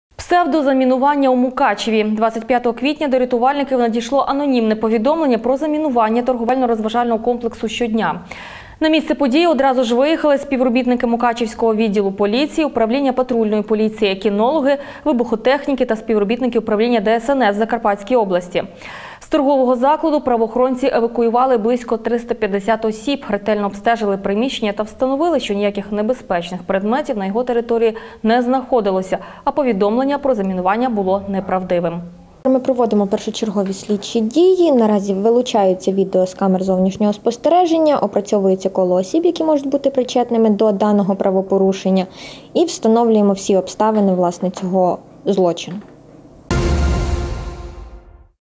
Псевдозамінування у Мукачеві
Псевдозамінування у Мукачеві. 25 квітня до рятувальників надійшло анонімне повідомлення про замінування торговельно- розажального комплексу Щодня. На місце події одразу ж виїхали співробітники Мукачівського відділу поліції, Управління патрульної поліції, кінологи, вибухотехніки та співробітники Управління ДСНС в Закарпатській області. З торгового закладу правоохоронці евакуювали близько 350 осіб, ретельно обстежили приміщення та встановили, що ніяких небезпечних предметів на його території не знаходилося, а повідомлення про замінування було неправдивим.